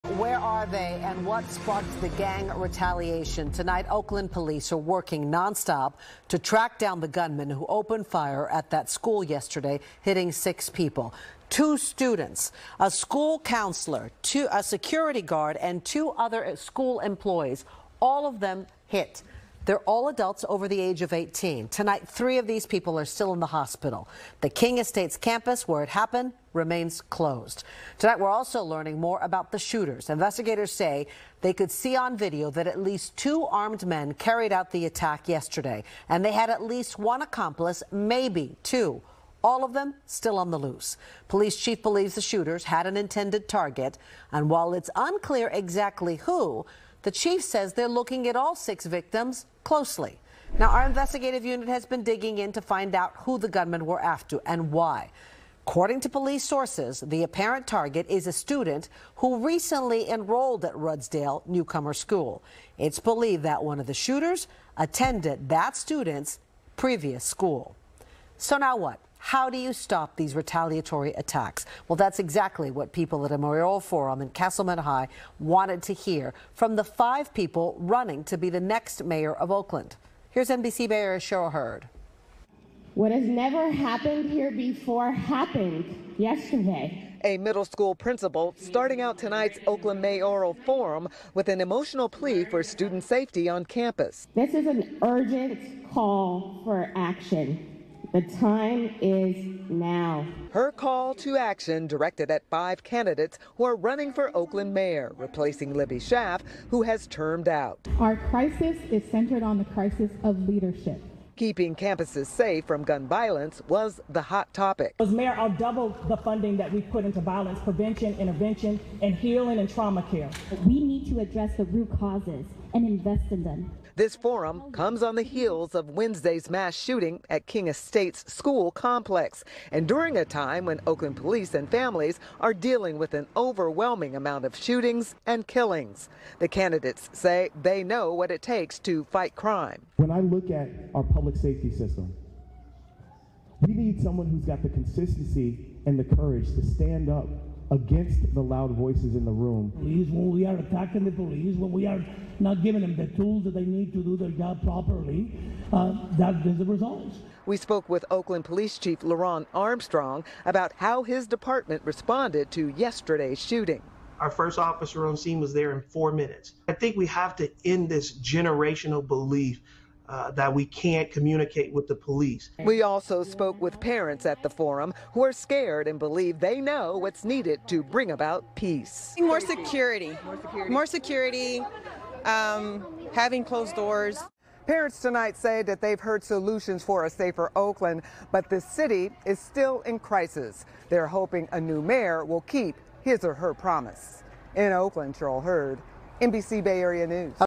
Community Calls for Action After Oakland School Shooting
Following a school shooting, some Oakland residents are wondering if the streets will ever become safe again. Cheryl Hurd reports.